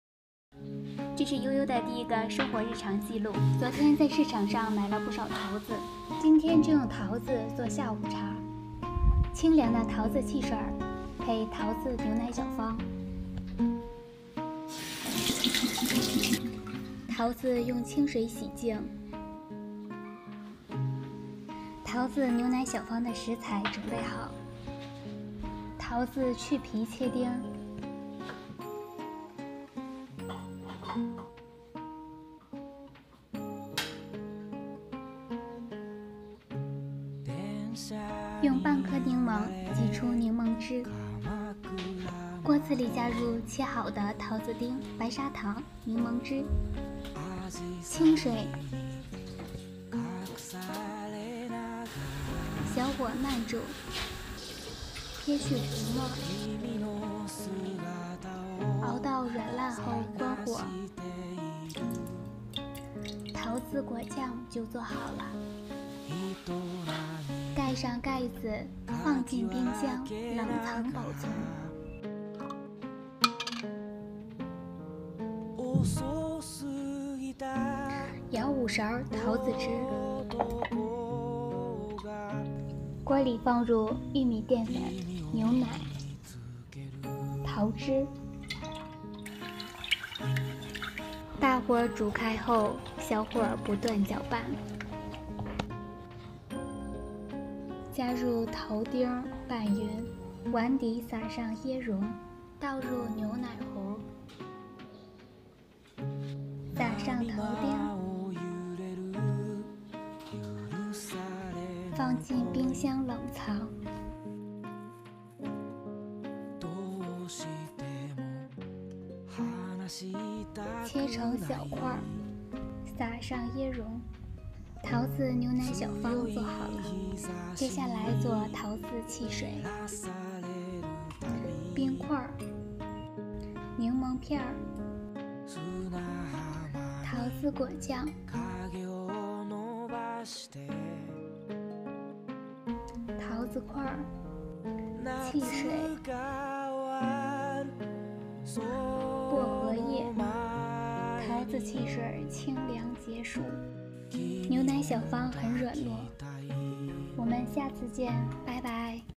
vlog|夏日下午茶清凉桃子汽水&牛奶小方！
桃子汽水真的超级超级好喝！强烈推荐你们做点桃子果酱囤着冲汽水喝呀！